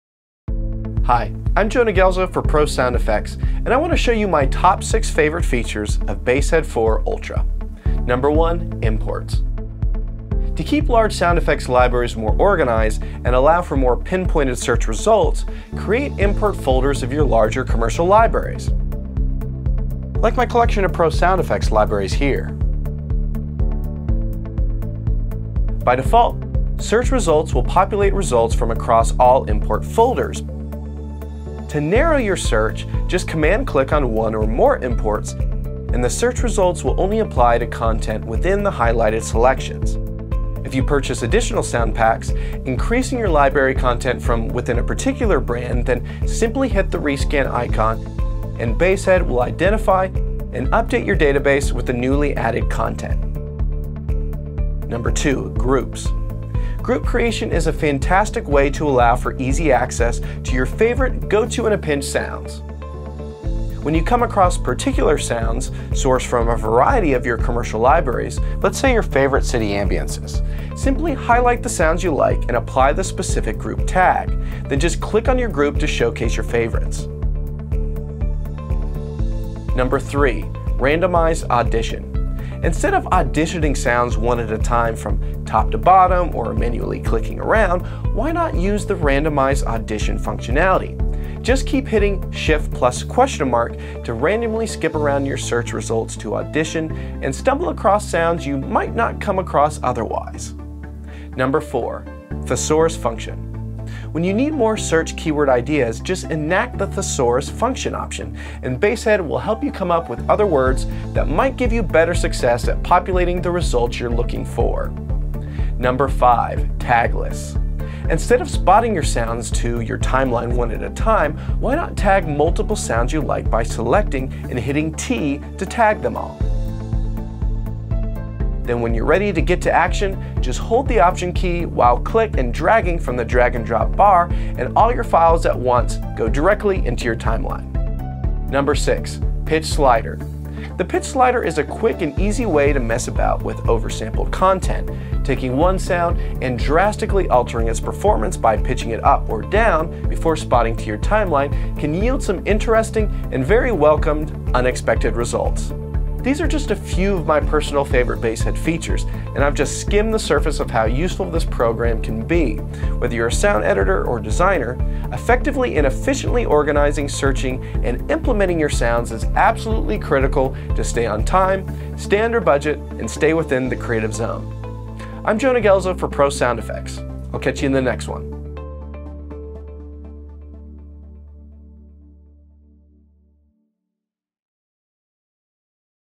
Top 6 BaseHead Features for Efficient Sound Editing Workflow
Whether you’re a sound editor or designer, effectively and efficiently organizing, searching, and implementing your sounds is absolutely critical to stay on time, under budget, and within the creative zone.

Jonah Guelzo (Sound Supervisor, Sound Designer, and Re-recording Mixer) shares his top 6 favorite features of Basehead 4.x Ultra:

1. Imports
2. Groups
3. Randomized Audition
4. Thesaurus Function
5. Tag Lists
6. Pitch Slider

These are just a few of Jonah's personal favorite BaseHead features which only skim the surface of how useful this program can be.